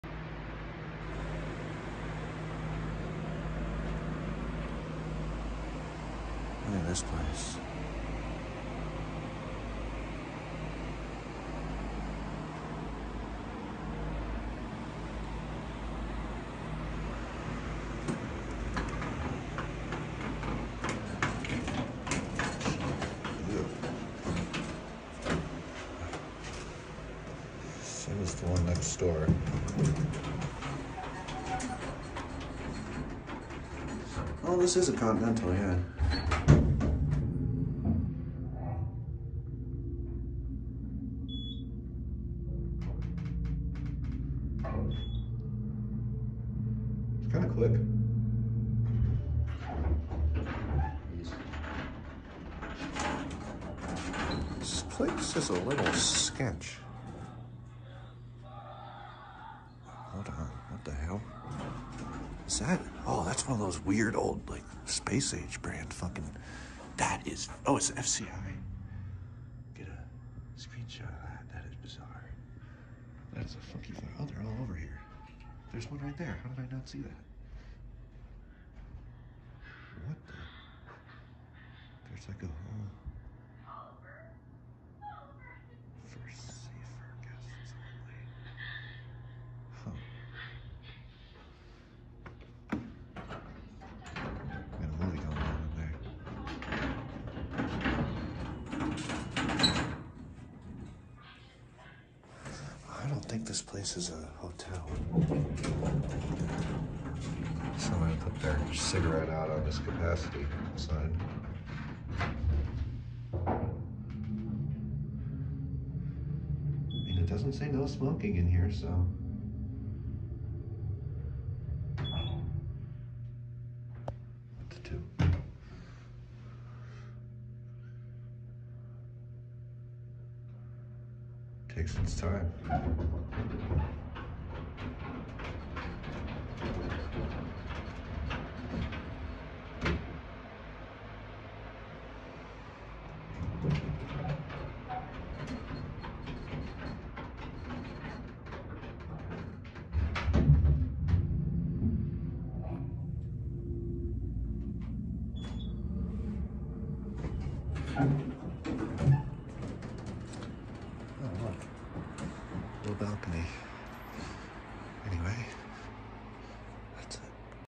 Sketchy Eltech Hydraulic Elevator @ GreenTree Inn - Centennial, CO.
TECH SPECS:
Brand: Eltech
Model: N/A
Drive: Hydraulic
Year Installed: 1991
Floors Served: 3
Capacity: 2500 lbs
Speed: 125 FPM
Doors: Single Slide

FIXTURES:
Buttons: Innovation Universal
Indicator: CE DH220
Lanterns: Innovation Arrow